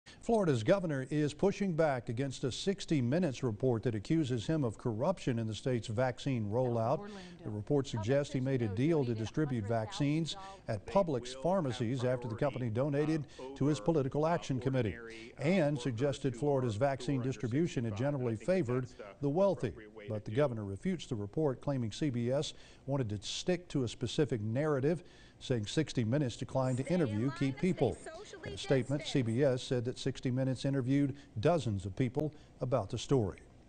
VIDEO: Florida’s governor refutes “60 Minutes” report that alleges corruption in state’s vaccine rol
VIDEO: Florida’s governor refutes “60 Minutes” report that alleges corruption in state’s vaccine rollout